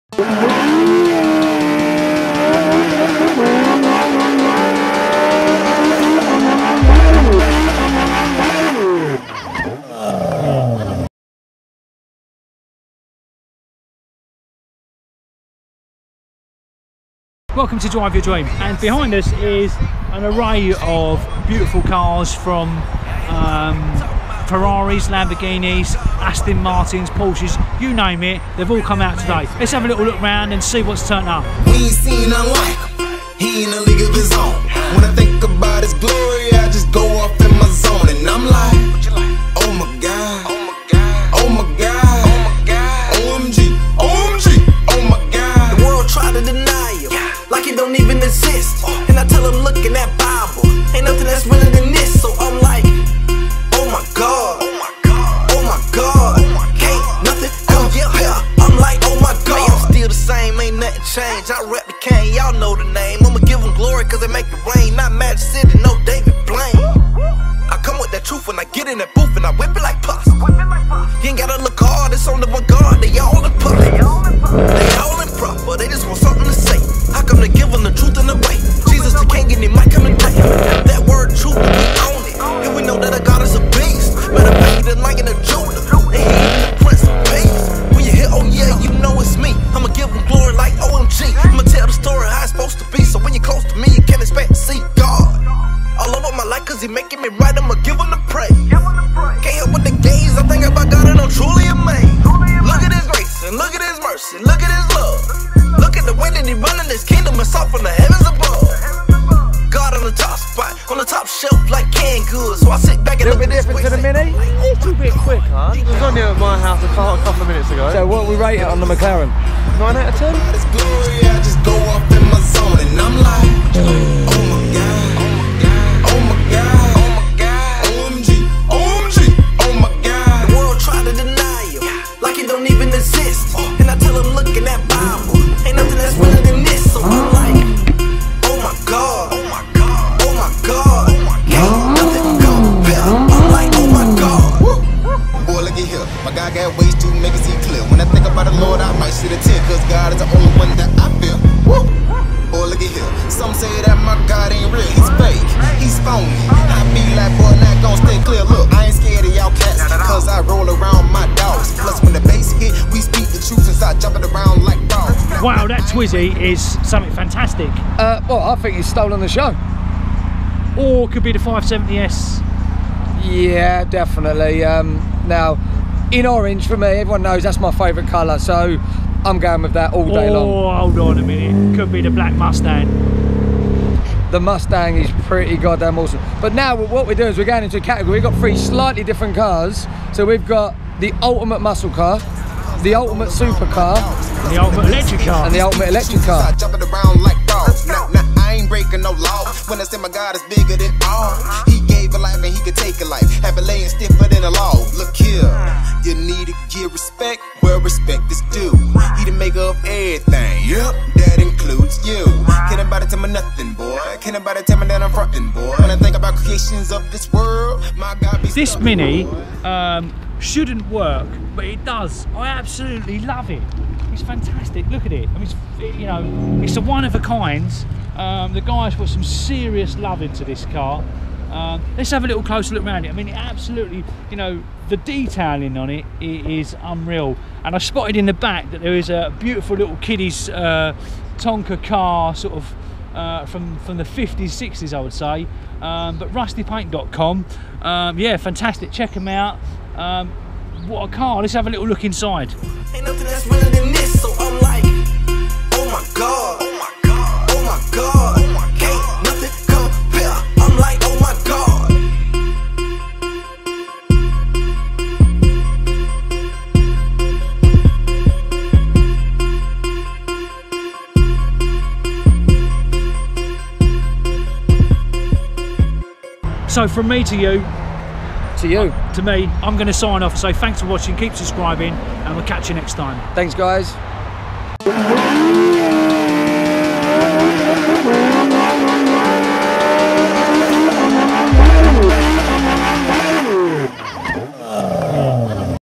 Supercar Meet at the Dick Turpin
Some fantastic cars and the Essex car meet, including a rusty mini which James of course loved.